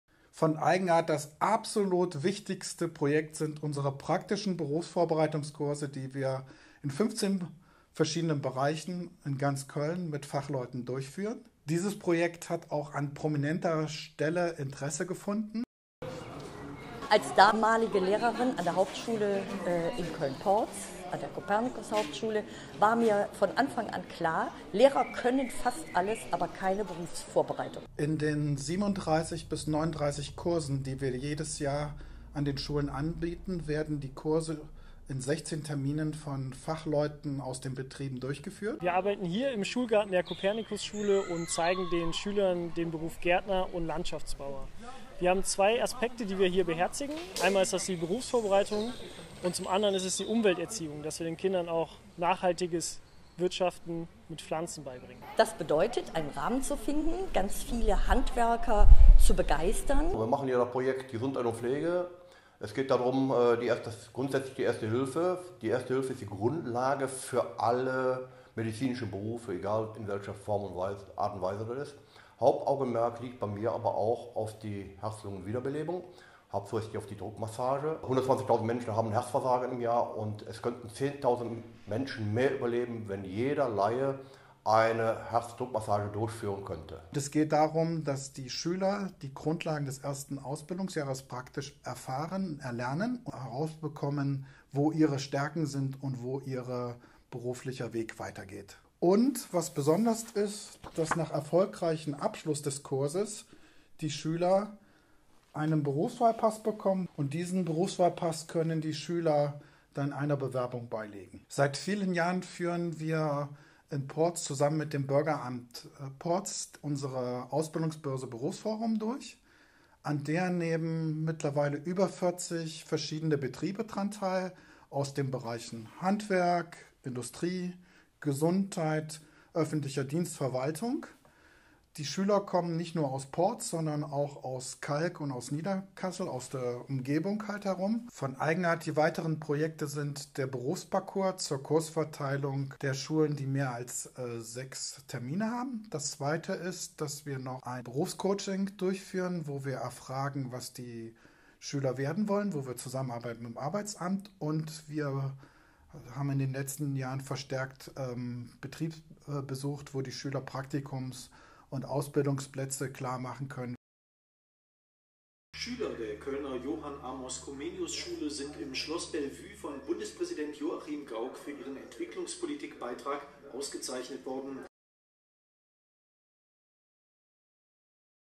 Berufsvorbereitung an Schulen: Was ist und macht Eigenart e.V. aus Köln-Porz?
Berurfsvorbereitungkurse an Schule, Ausbildungsbörse, Unternehmensbesuche für Schüler*innen und viel praktisches Wissen. Was Eigenart e.V. in Köln an Schulen anbietet.

Eigenart bietet praktische Berufsvorbereitung an Schulen in Köln: BerufsCHANCE. Und ist Ausrichter der Stadtteilausbildungsbörse BerufsFORUM.

Ein Video von thecunecopyrighted - powered by enqoozee.